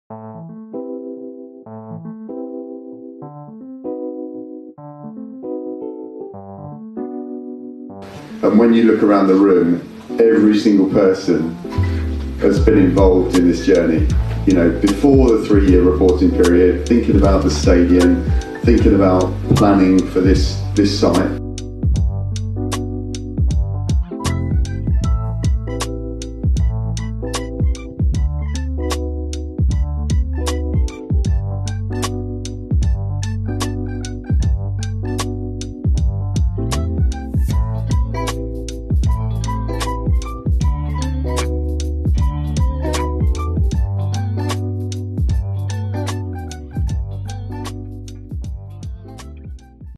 143 Million Pounds - Celebrating our latest Social Impact Value study!
143 Million Pounds 🤩

The headline figure of our latest Social Value Study for Brentford FC and Brentford FC Community Sports Trust 🐝

See behind the scenes of our launch event on Wednesday evening 🎬